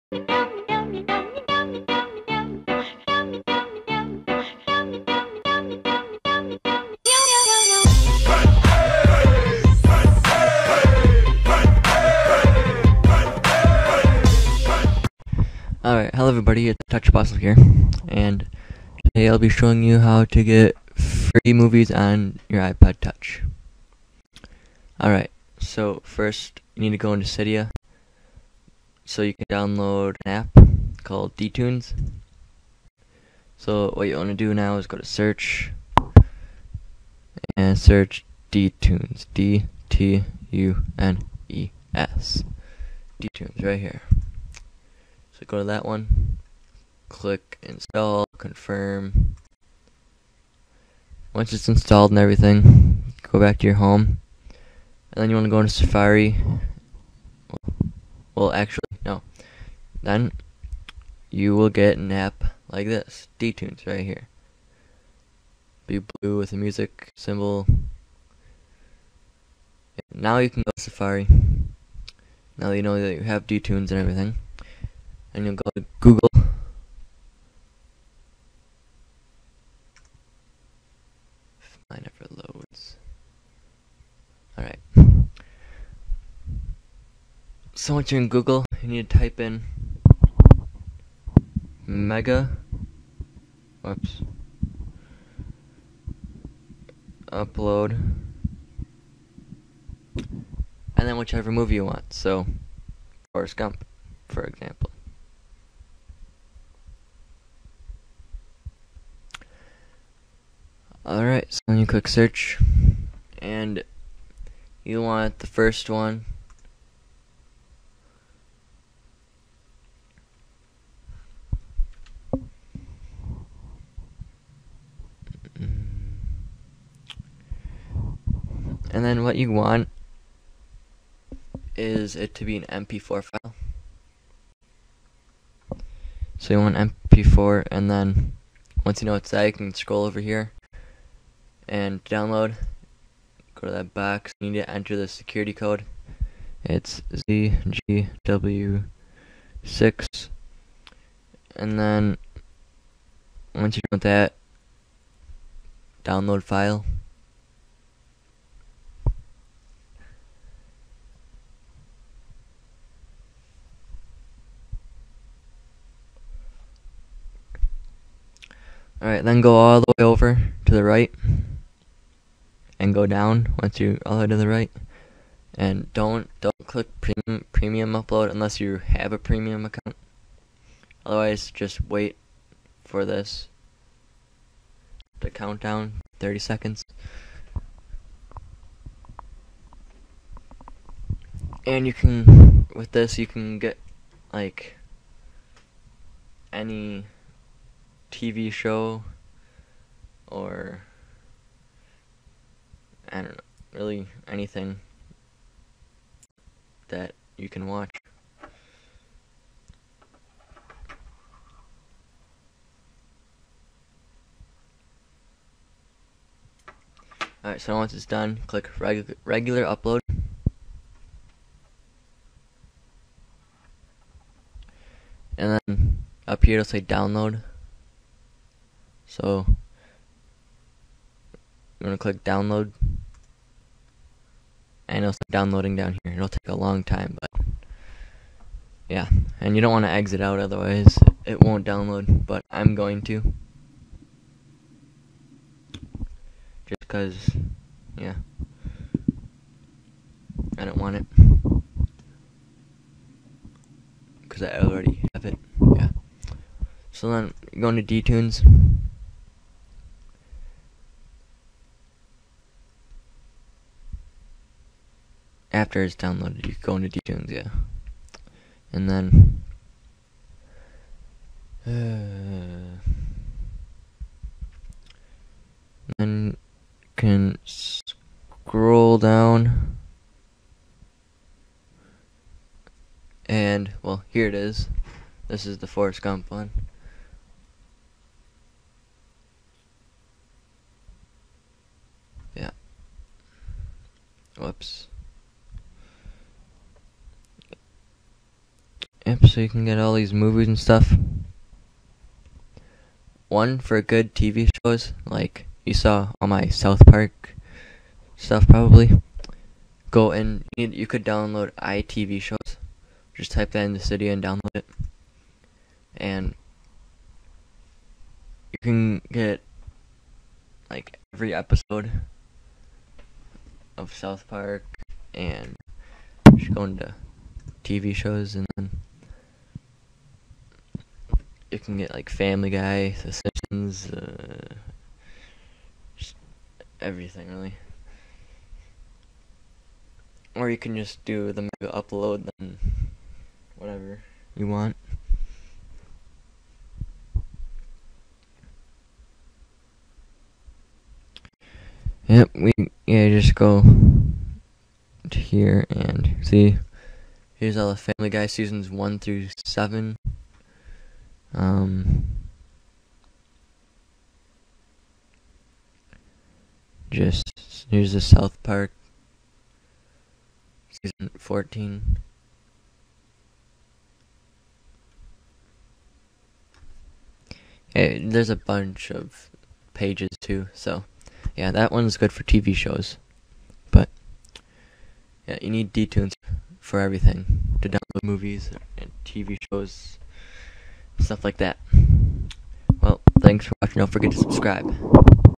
How to get free Movies on iPod Touch
this tutorial shows how to get free movies on ipod touch.
go to google on safari (on your ipod touch) and type in "megaupload__________"(whatever movie you want.
good tv shows app to download from cydia is iTVShows.